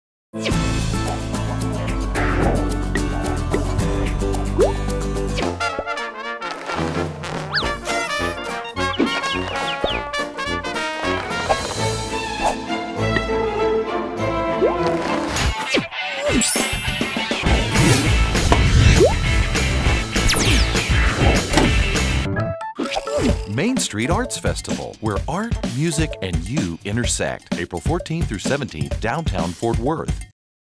2011 MAIN ST. Commercial...starring: Mona Lisa
Created by the uber-creative types at Concussion and Pinkerton Design (aka Khris Kesling and Gladys Pinkerton), our :30 spot was masterfully produced by CRM Studios, formerly of Fort Worth and now keeping very busy in Irving.  Enjoy!